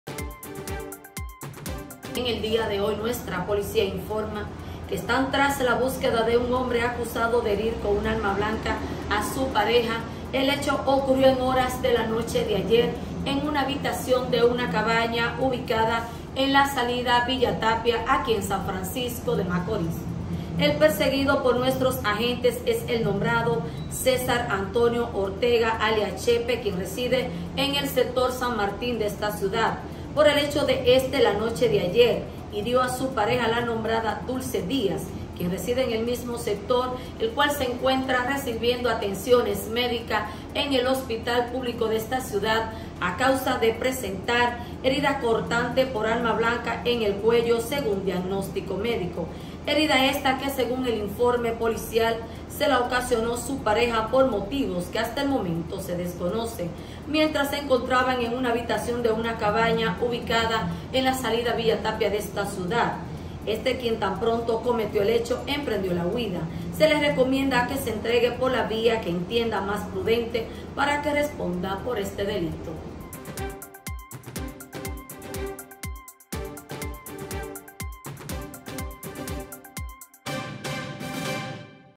Hombre hiere a puñaladas a su pareja sentimental en cabaña de SFM
SAN FRANCISCO DE MACORÍS. - Un hombre hirió de gravedad a su pareja sentimental mientras compartían en la habitación de una cabaña en la salida a Villa Tapia en esta ciudad.

Se trata del nombrado César Antonio Ortega alias Chepe, residente en el sector San Martín de esta ciudad, quien hirió en el cuello a su pareja sentimental la nombrada Dulce Díaz residente en el mismo sector.

Según el parte policial alias Chepe tras cometer el hecho emprendió la huida.

La policía activa la búsqueda del hombre para someterlo a la acción de la justicia.